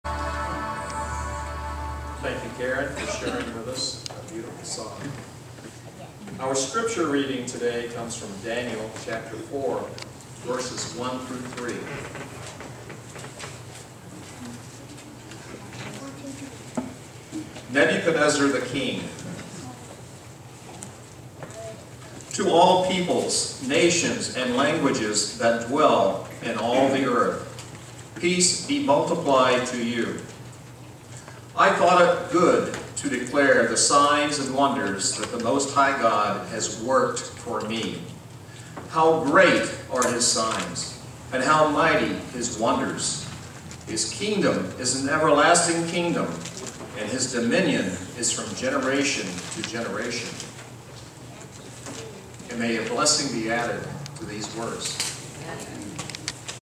NSBSDA - Scripture Reading - February 23, 2019
Presented By: Carl
Scripture: Daniel 4:1-3
New Smyrna Beach Seventh Day Adventist Church